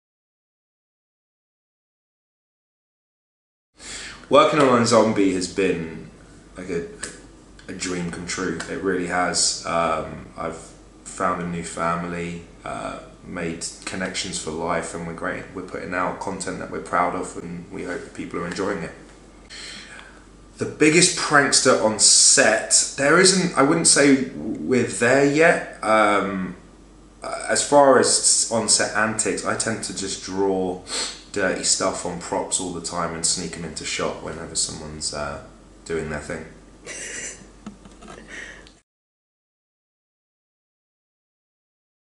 Rahul Kohli talks working on The CW' iZombie
Screen Tests w/ the phenomenal Rahul Kohli talking about working on The CW' iZombie & about his sneaky antics on set! Catch the epic Season Premiere tonight at 9/8c only on The CW!

Photographed / Filmed by Brad Everett Young
DreamLoudOfficial.com

Instagram.com/Bradley206
Twitter.com/BradEYoung